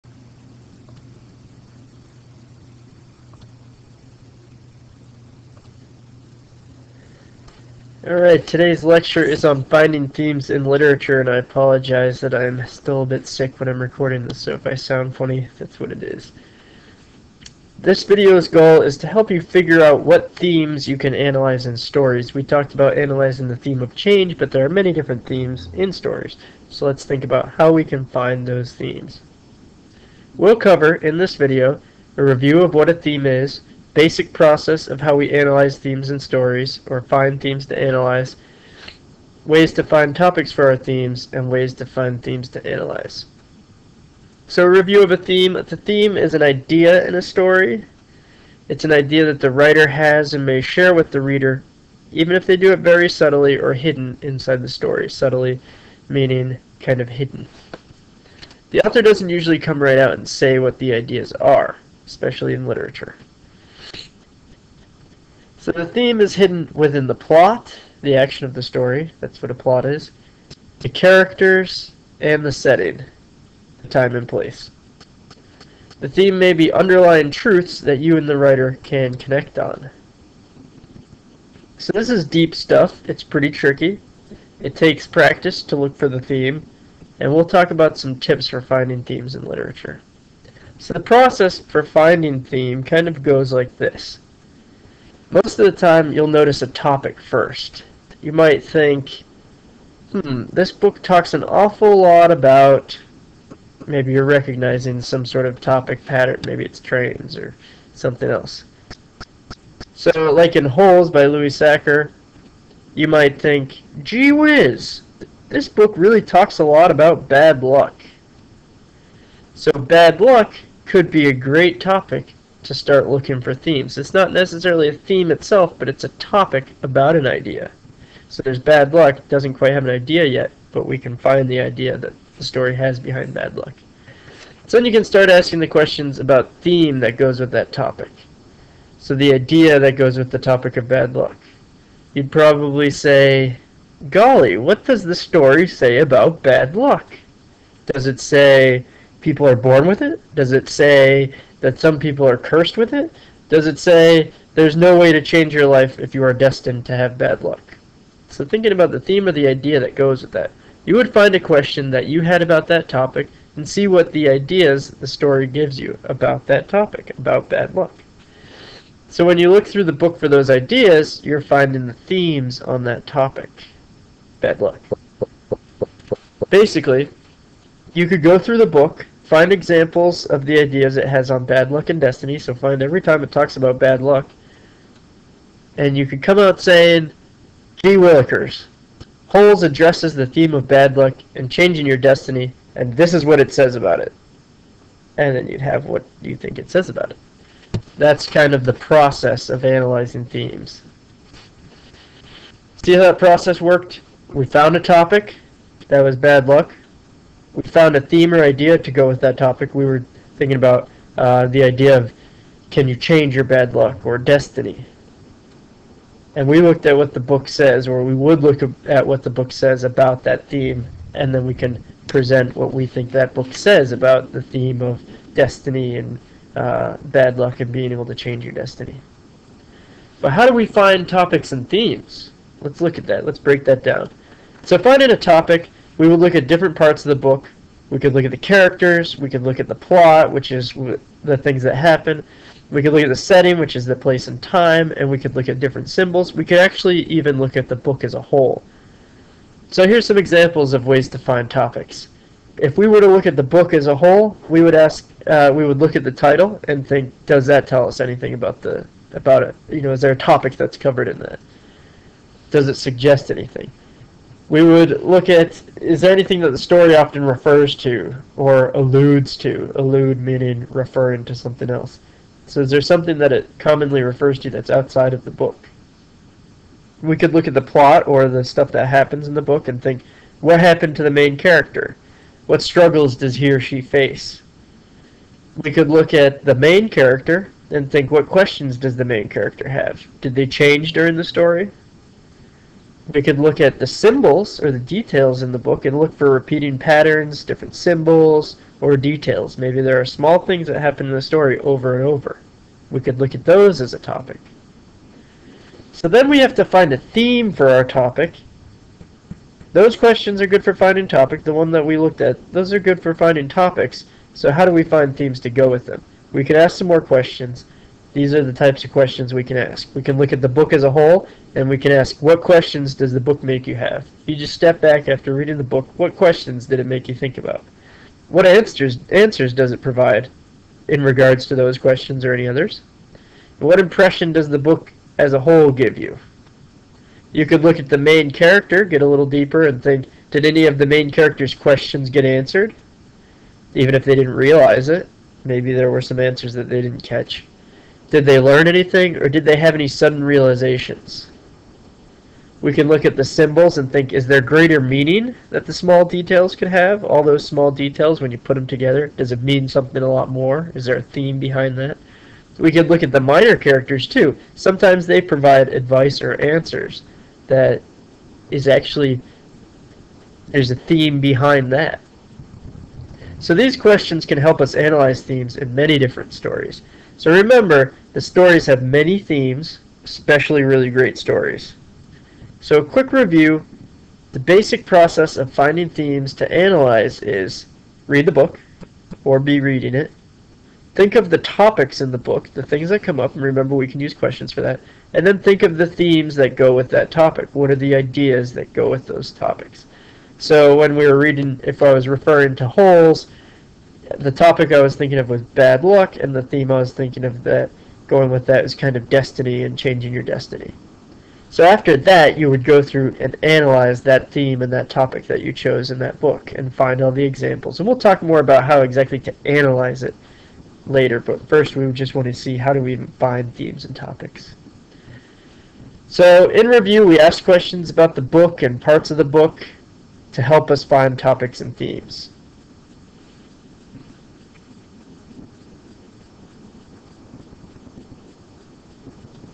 Finding Themes to Analyze in Literature
Today's video goes into Analyzing Themes in Literature
We'll cover:
• Review of what a theme is
• Basic overview of how we analyze themes in stories
• Ways to find topics for our themes
• Ways to find themes to analyze

Theme is an idea in a story. It is an idea that the writer has and may share with the reader, very subtly, or hidden inside the story. The author doesn't usually come right out and say what the ideas are.
The theme is hidden within the plot (action of the story), the characters, and the setting (time and place).
The theme may be underlying truths that you and the writer can connect on. This is deep stuff. It takes practice to look for theme. We'll talk about some tips for finding themes in literature.

Most of the time, you'll notice a topic first.
You might think, "hm, this book talks an awful lot about _________"
Like in Holes by Louis Sachar, you might think, "Gee wiz, this book really talks a lot about bad luck."
Bad luck, could be a great topic to start looking for themes.
Then you can start asking questions about the THEME that goes with that topic.
So, the IDEA that goes with the topic of bad luck.
You'd probably say "Golly, what does the story say about bad luck?"
Does it say people are born with it? Does it say that some people are cursed with it? Does it say there is no way to change your life if you are destined to have bad luck?
You would find a question that you had about that topic, and see what ideas the story gives you. When you look through the book for those ideas, you're finding the THEMES on that topic (bad luck). Basically, you could go through the book and find examples of the ideas it has on bad luck and destiny, and you could come out saying "Gee willickers, Holes addresses the theme of bad luck and changing your destiny, and this is what it says about it. "

Ways to find topics:
The Book
Look at the title -- Does it suggest anything?
Is there anything the story often refers to (or alludes to)?

The Plot
What happened to the main character?
What struggles does he or she face?

The Main Character
What questions does the main character have?
Did they change during the story?

The Symbols
Look at repeating patterns, symbols, or details.

Ways to find themes for your topic:
The Book
What questions does the book make you have? What answers does it provide?
What impression does the book as a whole give you?

The Main Character
Did any of the main character's questions get answered (even if they didn't realize it)
Did they learn anything?
Did they have any sudden realizations?

The Symbols
Is there greater meaning that the small details could have?

The Minor Characters
Do they provide any advice or answers?